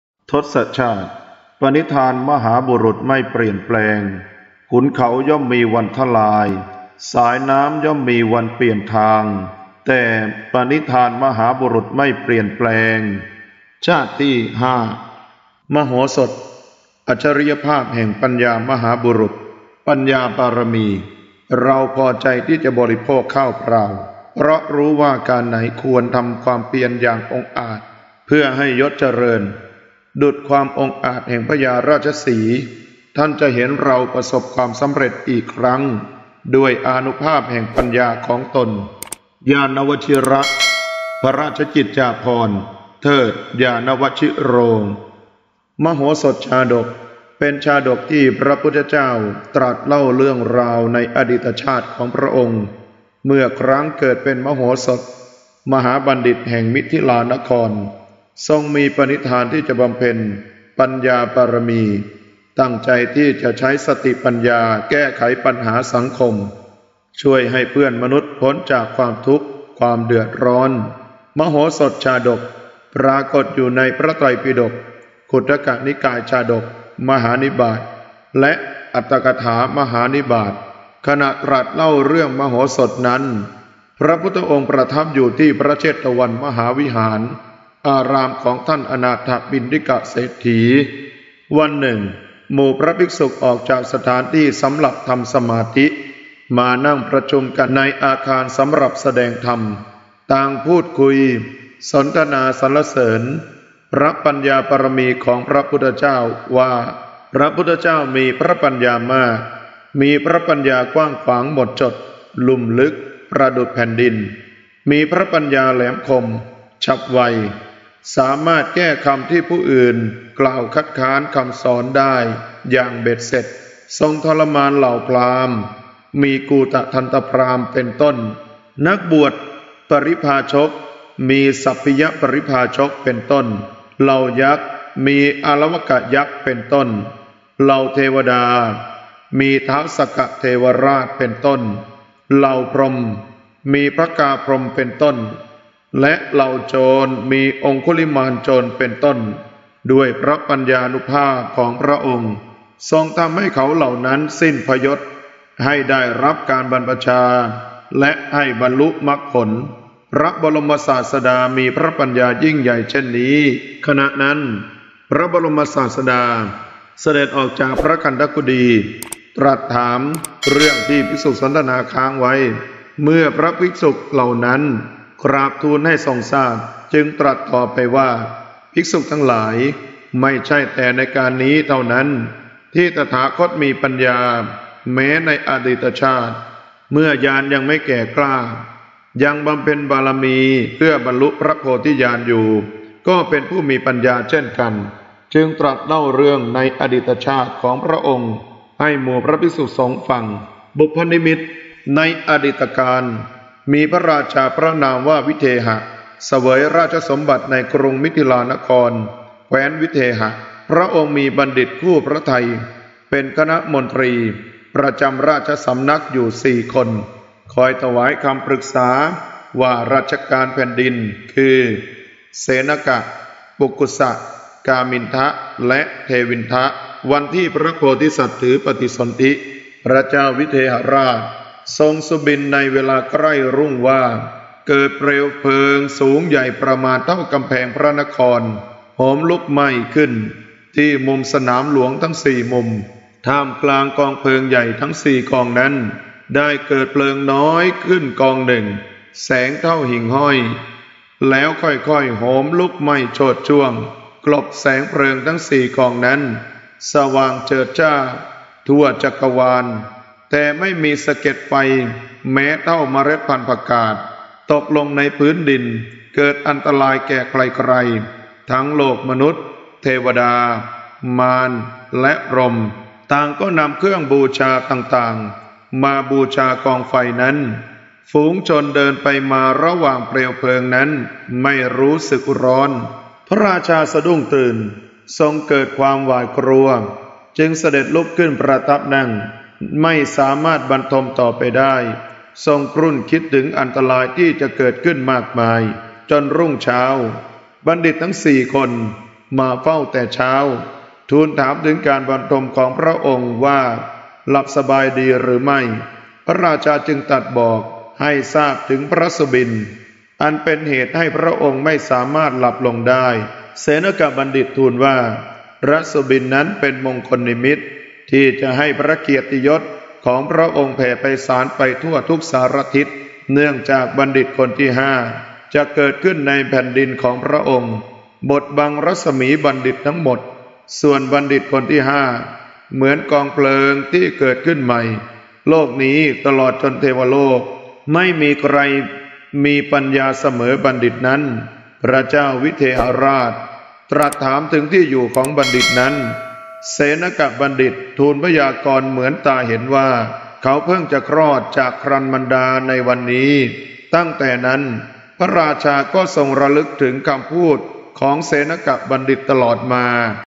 ทศชาติ 036 มโหสถ อัจฉริยภาพแห่งปัญญามหาบุรุษ
ทศชาติเป็นเรื่องราวในอดีตชาติของพระพุทธเจ้า ขณะที่พระองค์กำลังบำเพ็ญบารมีเพื่อบรรลุพระโพธิญาณ และเป็นเรื่องราวทางพระพุทธศาสนาที่ชาวโลก ไม่เฉพาะแต่คนไทย รู้จักมาที่สุดเรื่องหนี่ง บรรพบุรุษไทยในอดีต ได้นำทศชาติมาเขียนเป็นภาพจิตรกรรมฝาผนังพระอุโบสถตามวัดต่างๆ บอกเล่าเรื่องราวให้ลูกหลานได้ศึกษาเส้นทางชีวิตมหาบุรุษ ผู้ที่จะเกิดมาเป็นพระศาสดาเอกของโลก